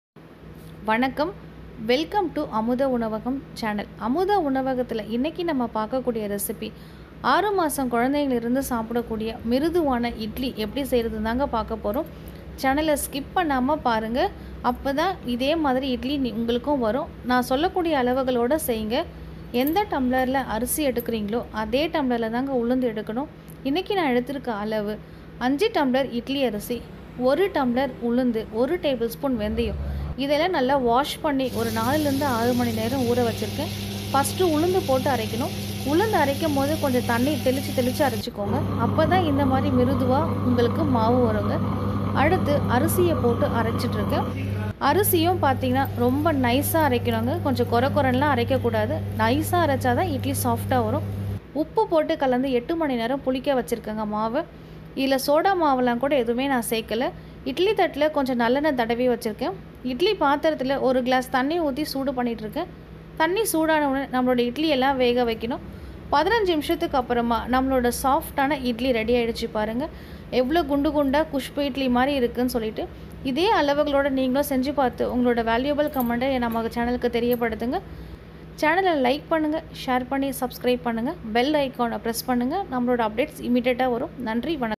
IDLY BATTER/SOFT IDLY BATTER/HOW TO MAKE SOFT IDLY BATTER/இட்லி மாவு
Ingredients :
IDLY RICE:5CUP(2.5CUP)
URAD DAL:1CUP(1/2CUP)
FENUGREEK:1tsp(1small tsp)